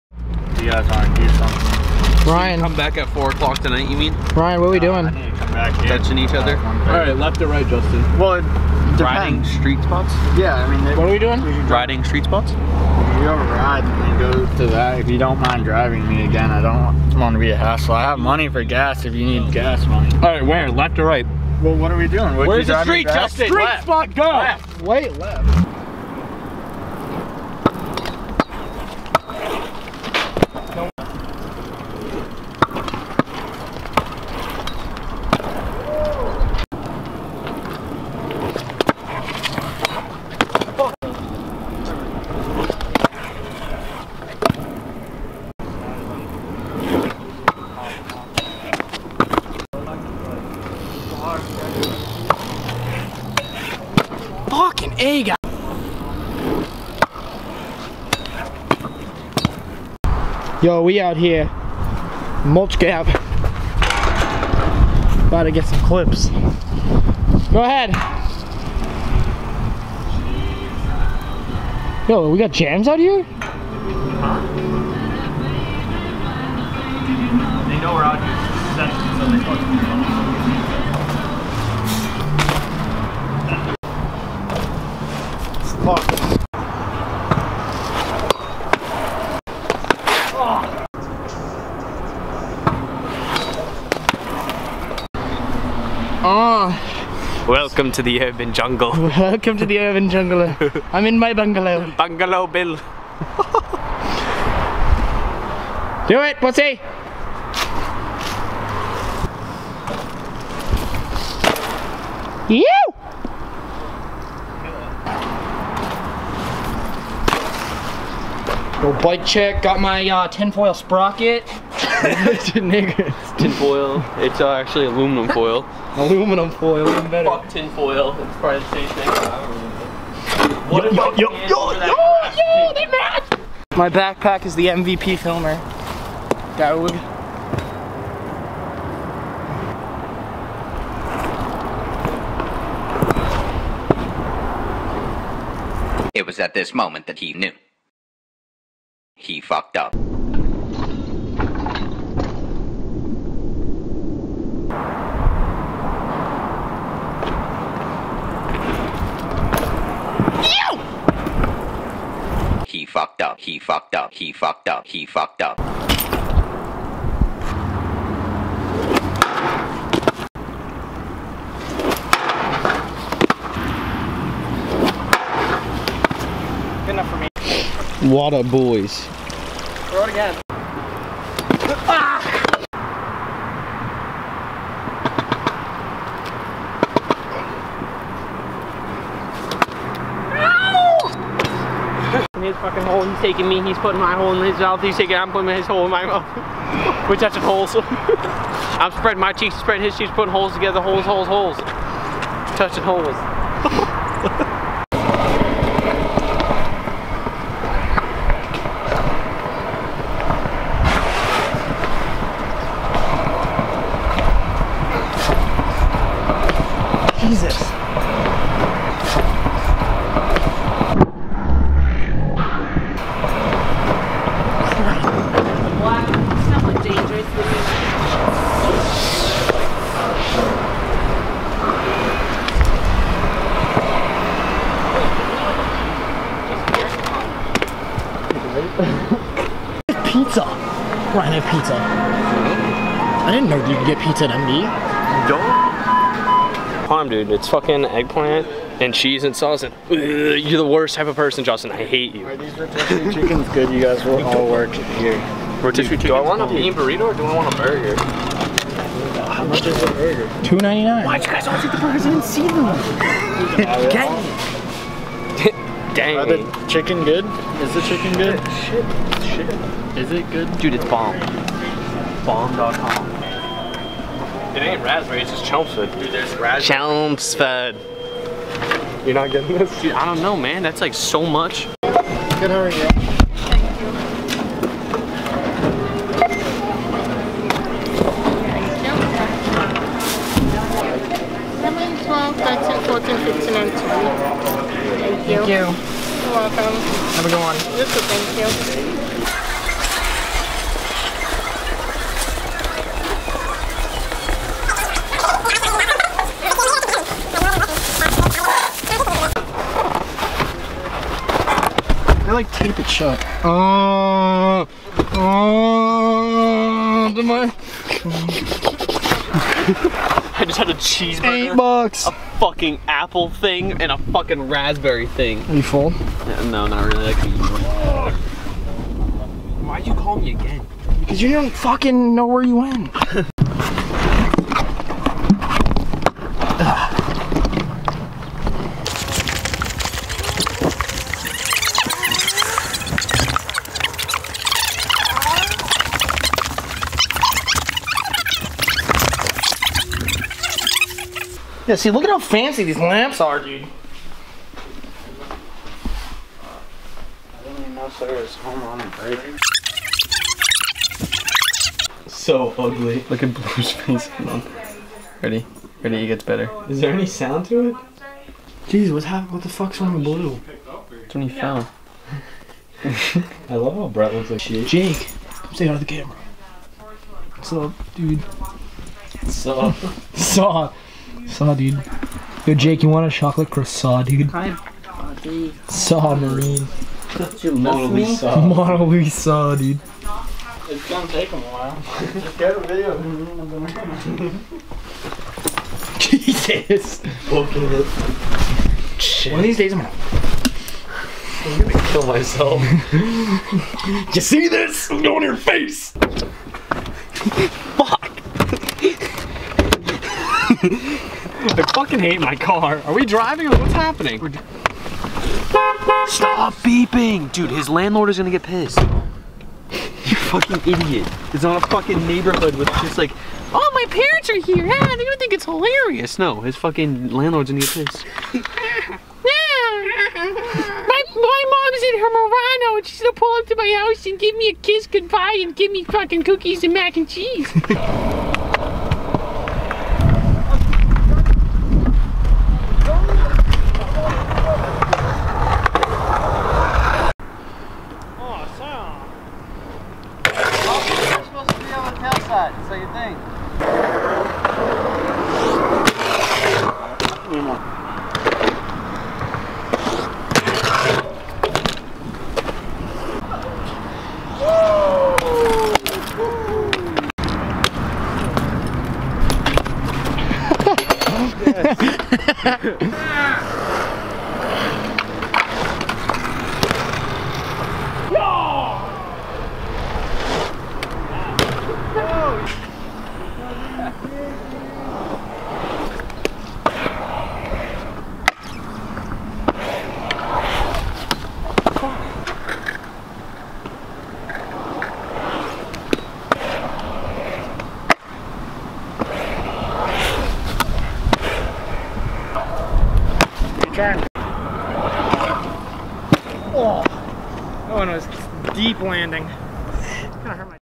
Webisode 2 - Holes and Jibbs
This is our 2nd webisode. We went riding around Amherst street and a local crap park. Let me know if you guys like these and we should keep making them! Sorry there is really no ending, the camera died.